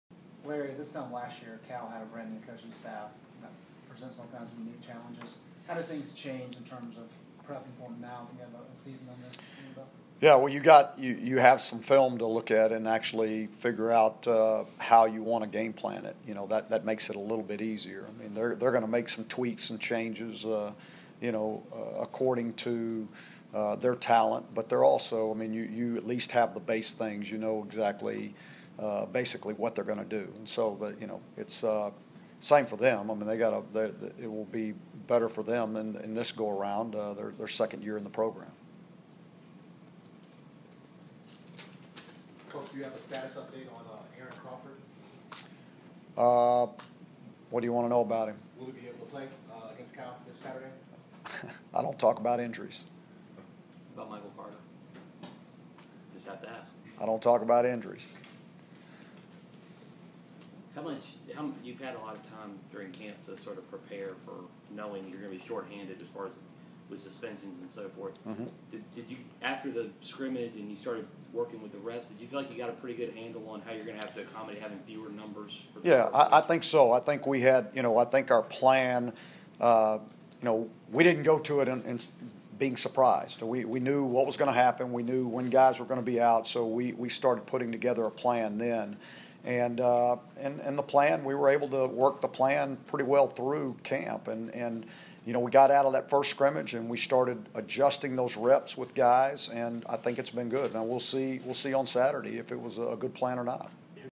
ICTV: Larry Fedora on Preparing for Cal
The head coach spoke to the media in advance of UNC’s opening game against Cal.